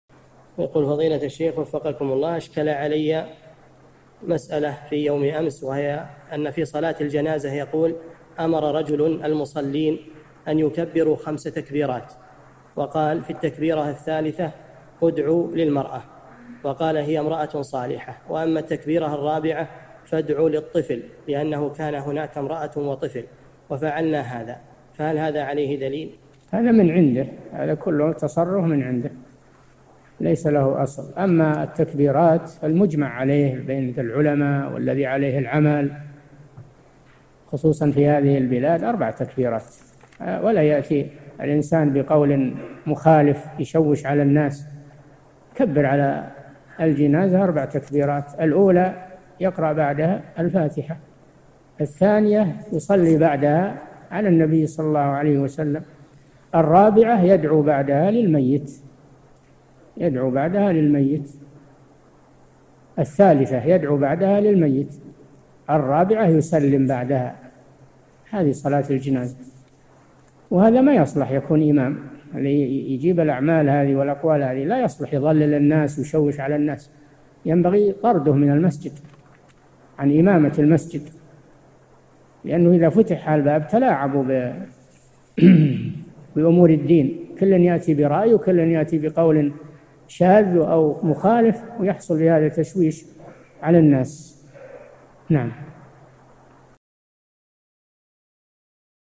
[1061 /1350]ماتت امرأة وفي بطنها جنين فأمر رجل المصلين أن يكبروا خمس تكبيرات وفي الثالثة يدعون...
-[1061 /1350] السؤال: أشكل علي مسألةٍ في يوم أمس، وهي: أن في صلاة الجنازة يقول: " أمر رجل المصلين أن يكبروا خمس تكبيراتٍ " وقال في التكبيرة الثالثة: " ادعوا للمرأة "، وقال: هي امرأة صالحة، وأما التكبير الرابعة فادعوا للطفل؛ لأنه كان هناك امرأة وطفل، وفعلنا هذا، فهل هذا عليه دليل؟
الجواب: هٰذَا من عنده، هٰذَا كله تصرف من عنده، ليس له أصل، أمَّا التكبيرات؛ المجمع عليه عند العلماء، وَالَّذِي عليه العمل -خصوصًا في هٰذِه البلاد- أربع تكبيرات، ولا يأتي الإنسان بقولٍ مخالف يشوِّش عَلَىٰ الناس، كبِّر عَلَىٰ الجنازة أربع تكبيرات:  الأولى: يقرأ بعدها الفاتحة. الثانية: يصلي بعدها عَلَىٰ النَّبِيّ صلى الله عليه وسلم . الثالثة: يدعو بعدها للميت. الرابعة: يسلِّم بعدها.هٰذِه صلاة الجنازة، وَهٰذَا ما يصلح يكون إمامًا، اللي يجيب الأعمال هٰذِه والأقوال هٰذِه لا يصلح يضلِّل الناس ويشوِّش عَلَىٰ الناس، ينبغي طرده من المسجد، عن إمامة المسجد؛ لأنَّه إذا فُتح الباب تلاعبوا بأمور الدين، كلٌّ يأتي برأيه، وكلٌّ يأتي بقولٍ شاذ أو مخالف ويحصل بهذا تشويش عَلَىٰ الناس.
للاشتراك في قناة الشيخ صالح الفوزان
أسئلة وأجوبة من شرح قاعدة جليلة ( 1350 ) مقطع

للاستفسار والإقتراحات والدعم
رئيس الفريق
لمشروع كبار العلماء